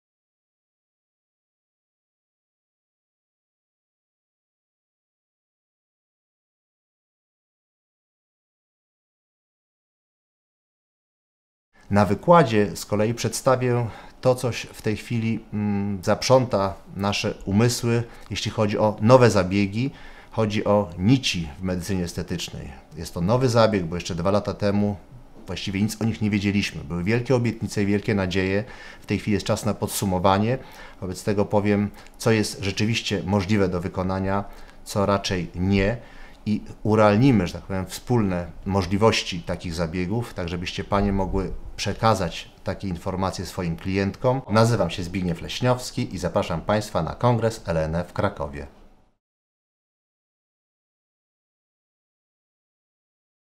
Dr Zbigniew Leśniowski zaprasza na 33. Kongres i Targi LNE
Lekarz medycyny estetycznej, Dyrektor Medyczny Centrum Szkoleniowego Kosmetologii Medycznej zaprasza na 33. Międzynarodowy Kongres i Targi LNE w Krakowie!
Bądźcie tam z nami!